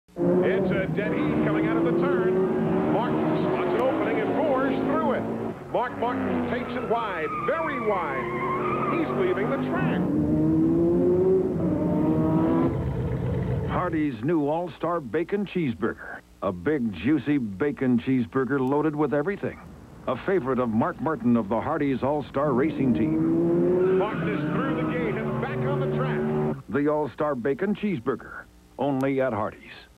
Hardee's Commercial w/ Mark Martin (1999)
Television commercial for Hardee's All Star Bacon Cheeseburger featuring NASCAR driver Mark Martin. (1999)